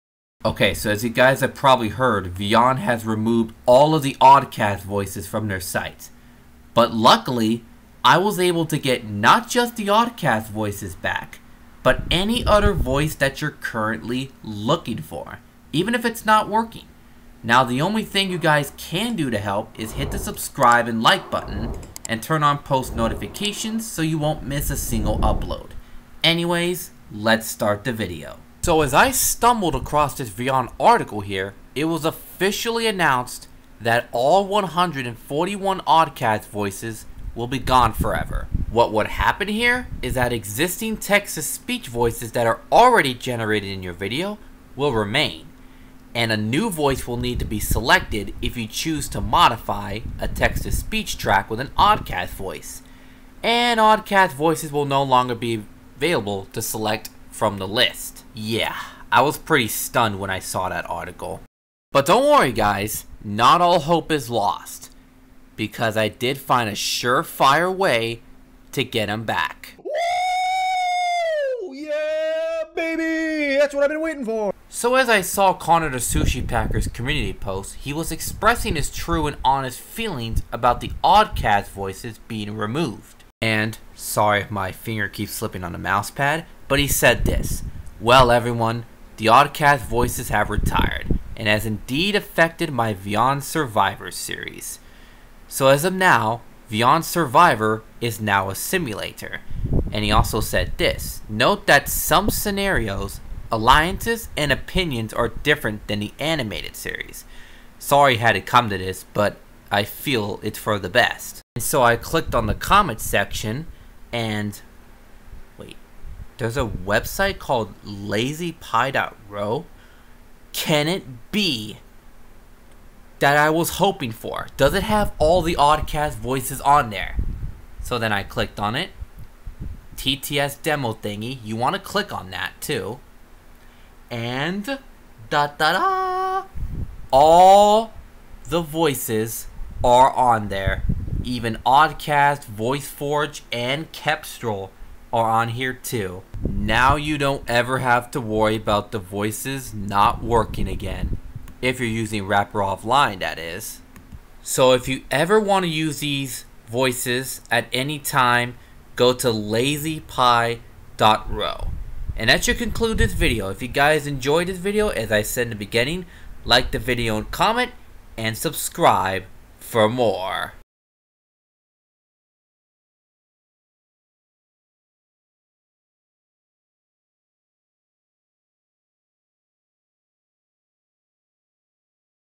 How to get Oddcast, Voiceforge, and Cepstral voices back on Vyond!
I hope this tutorial video helps. Such a shame that Vyond had to remove the oddcast voices.